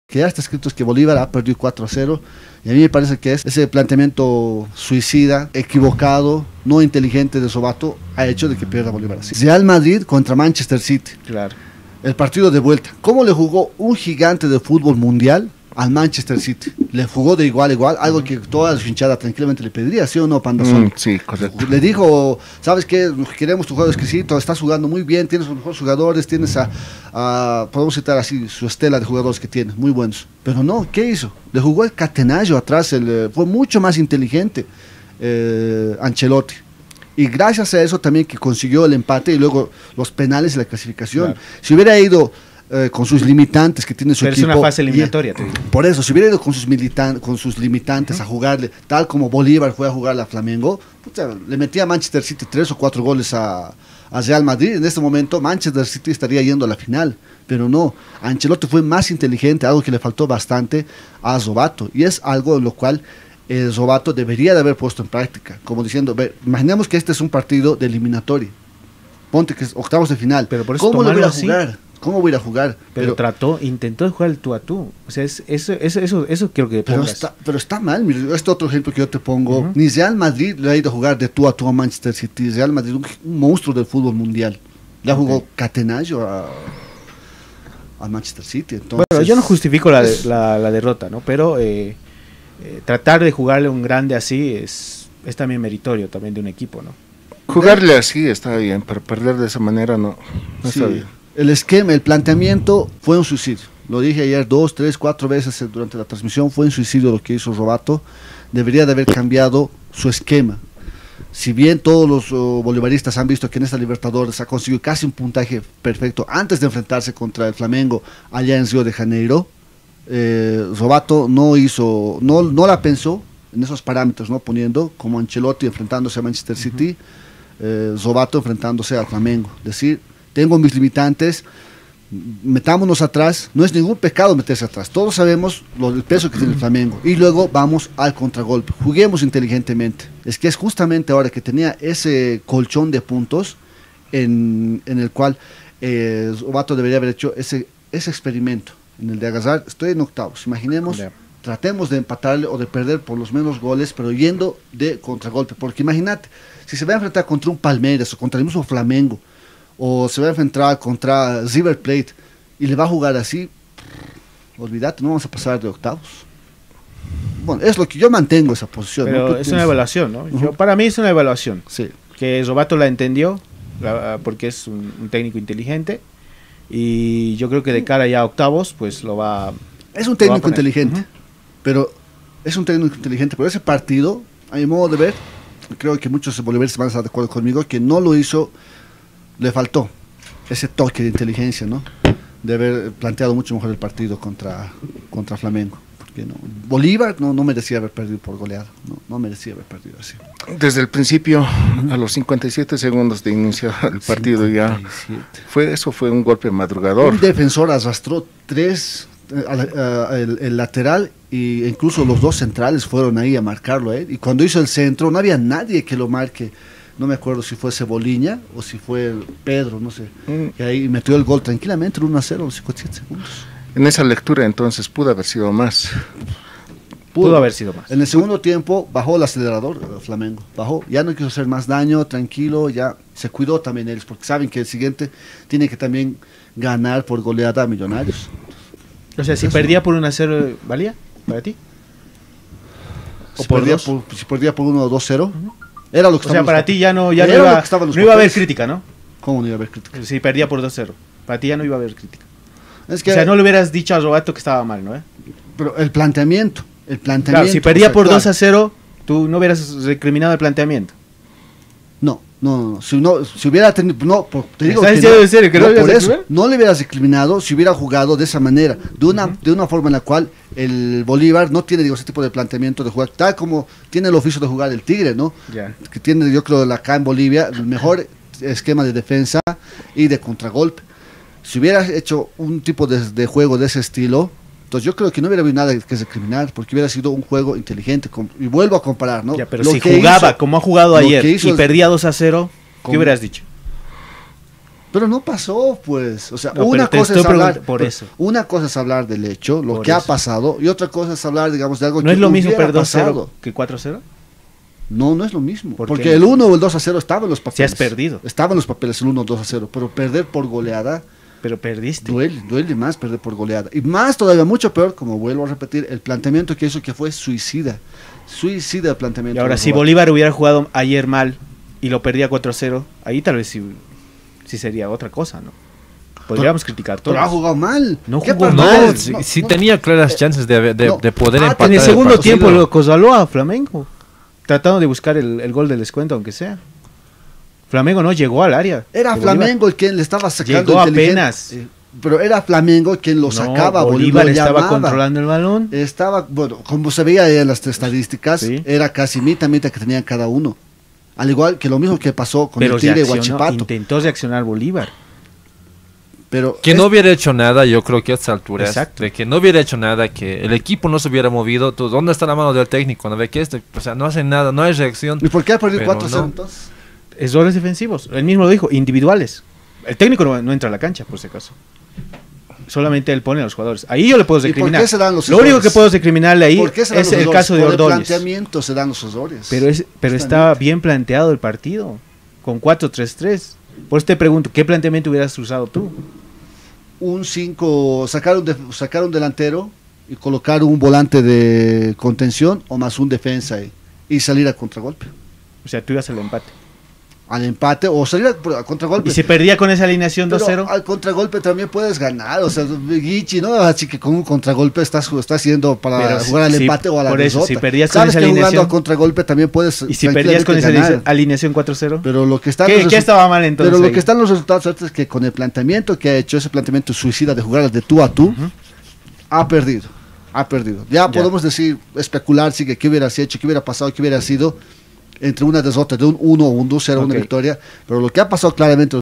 🔵⚪️🔵 BOLÍVAR FUE SU1C1D4 ANTE FLAMENGO Y POR ESO PERDIÓ 4 x 0
"RealMadrid no le jugó de igual a igual en su casa al ManchesterCity sino que le hizo un Catenaccio y lo mató de contragolpe, ESO DEBERÍA HABER HECHO EL Bolívar"
AL VIVO ESTADIO69 - 16/05/24
🔥🌟Mira el programa completo en ULUPICAST YOUTUBE 📱💻🖥📺📡